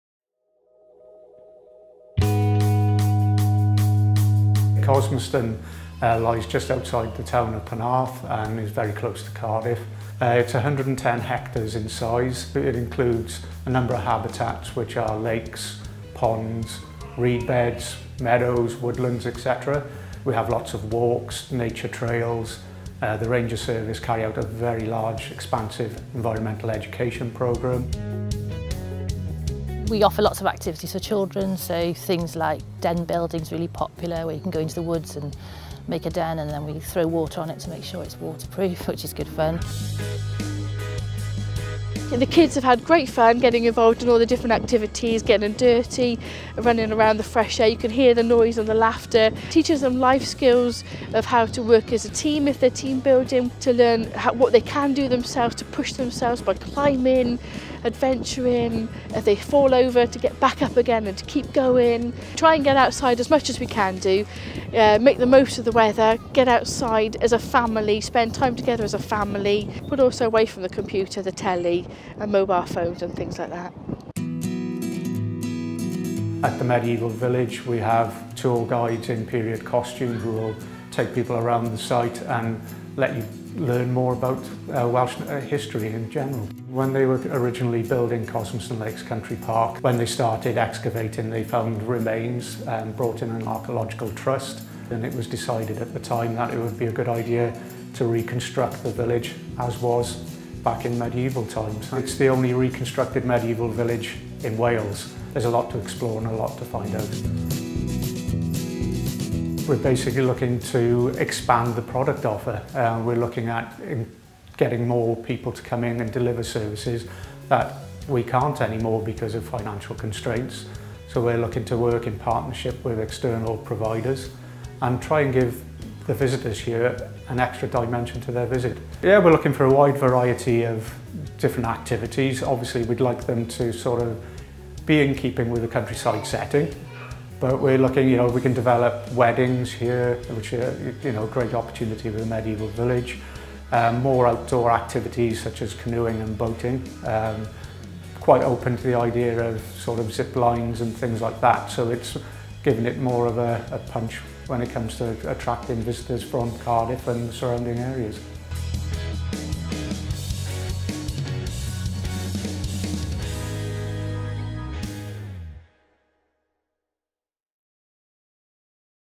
Cosmeston Lakes Country Park commercial opportunities available
The Vale of Glamorgan Council are looking to offer a range of commercial opportunities to businesses of all sizes. The park has over 250,000 visitors a year. It is situated just outside Cardiff and over 1.5 million people live within a 45 minute drive of the park.